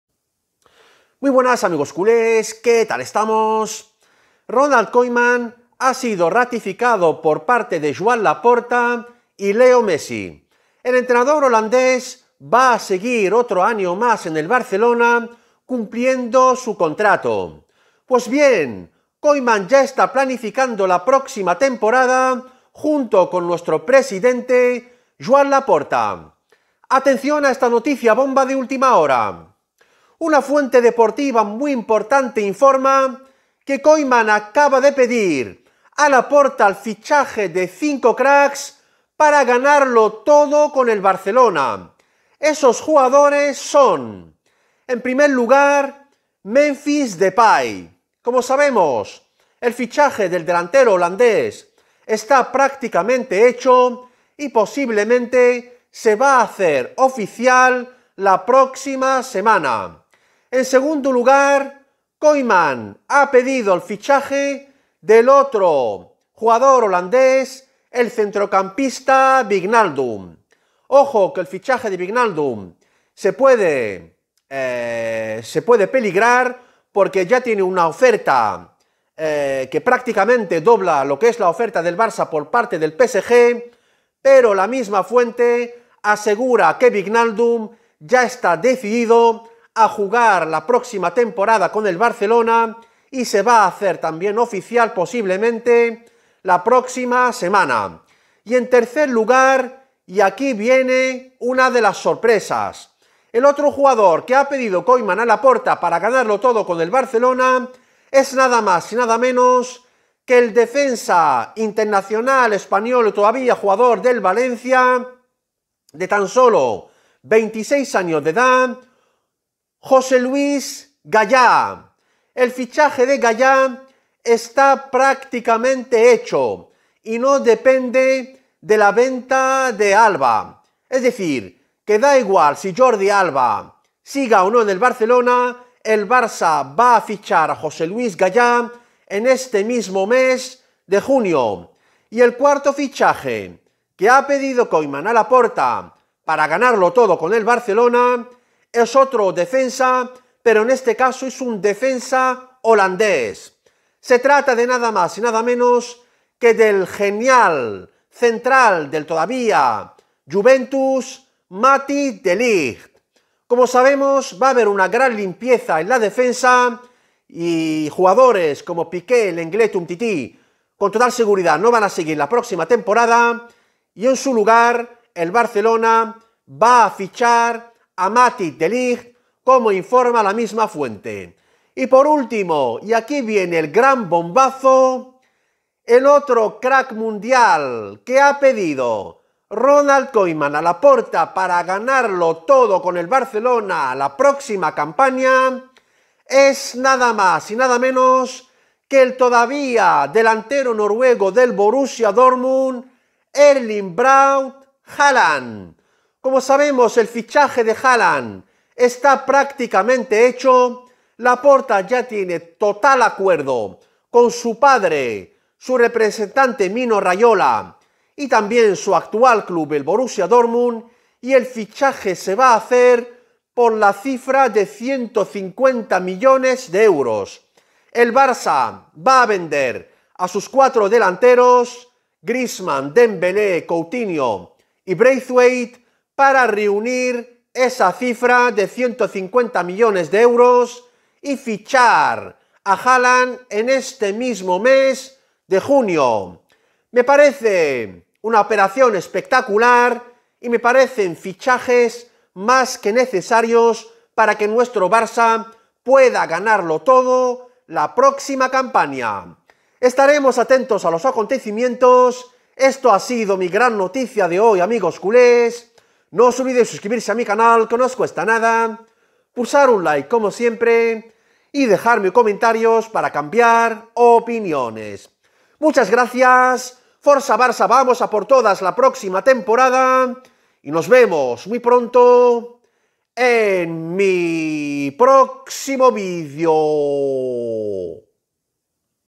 BOMBAZO: LOS 5 FICHAJES QUE ACABA DE PEDIR KOEMAN A LAPORTA PARA GANAR TODO CON EL BARCELONA 2021/22
KOEMAN ACABA DE PEDIR A LAPORTA EL FICHAJE DE CINCO CRACKS PARA GANARLO TODO CON EL NUEVO BARCELONA DE LA PRÓXIMA TEMPORADA